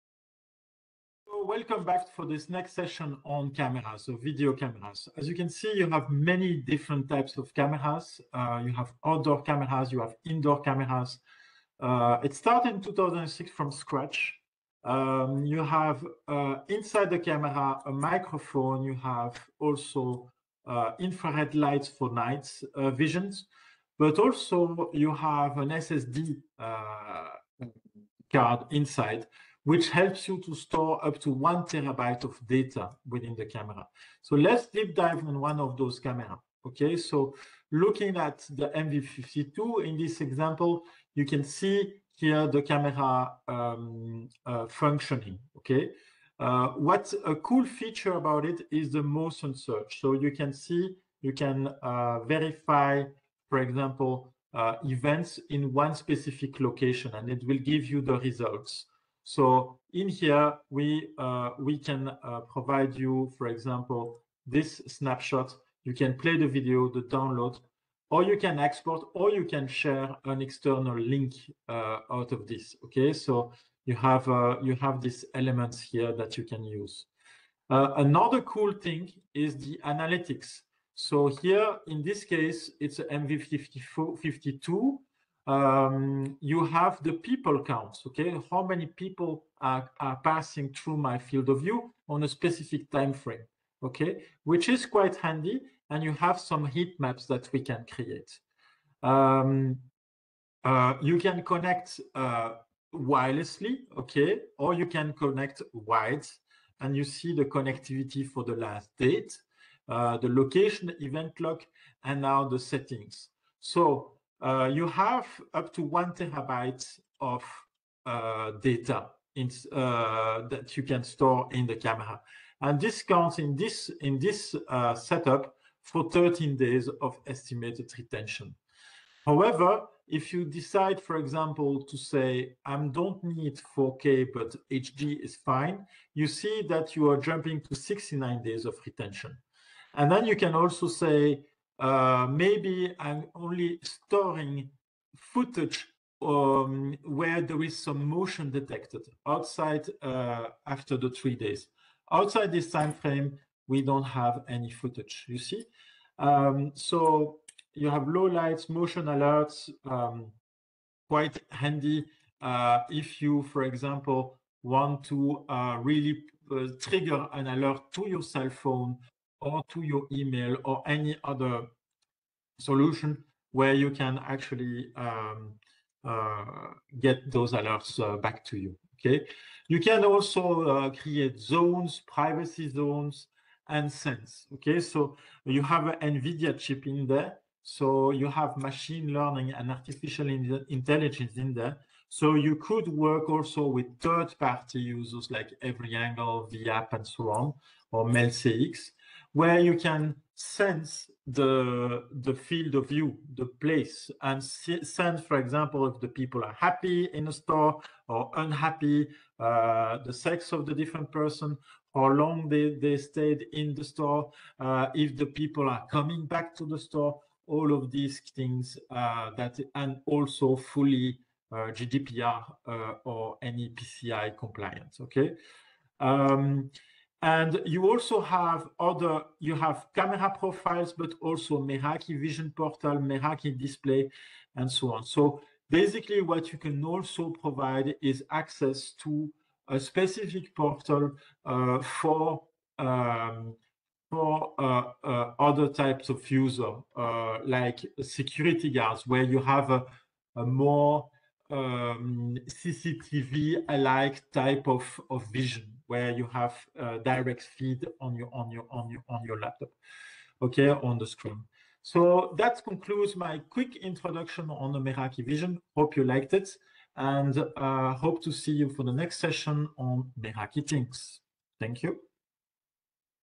A Quick demo introduction on Cisco Meraki MV Cameras
Quick Demo on Cisco Meraki Vision Cameras MV, Motion Search, People Count, Vehicle Count, Heatmaps...